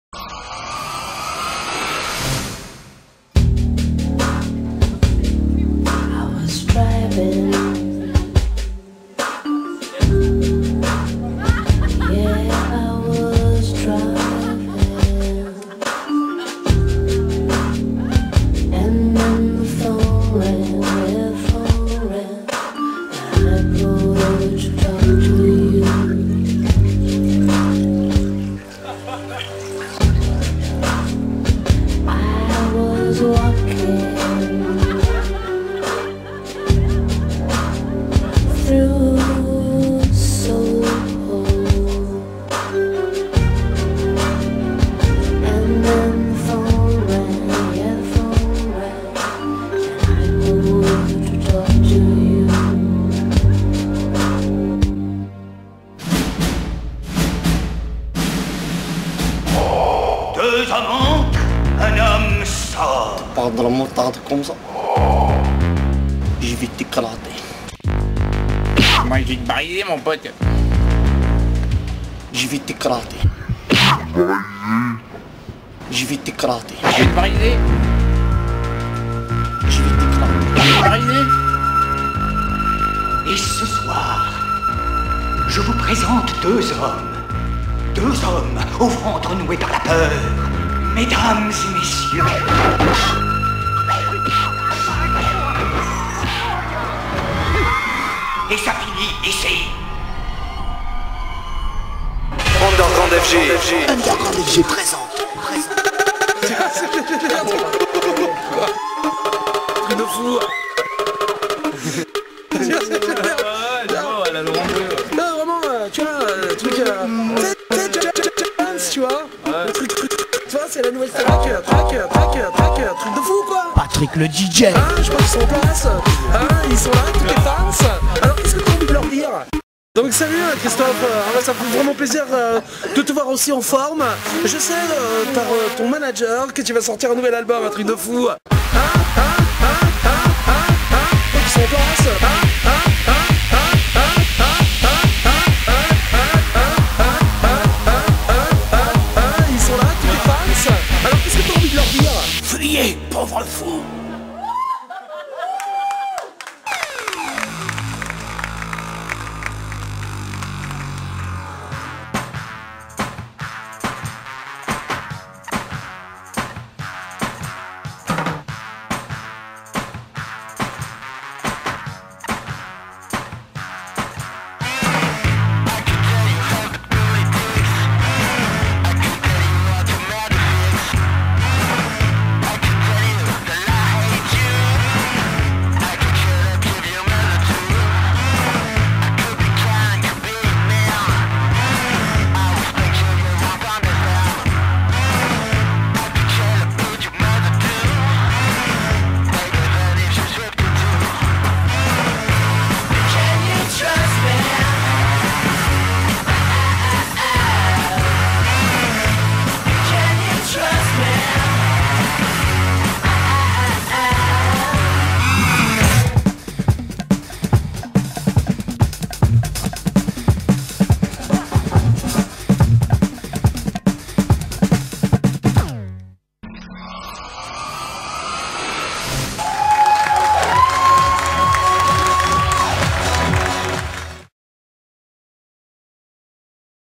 A LA RECHERCHE DE LA NOUVELLE TENTASTARAC'12
Episode 12 du Spectacle de fin d'année francophone, enregistré le mercredi 02 Juillet 2008, proposé par les élèves de l'école de Dj's Ucpa - AMS 21 à Lyon - France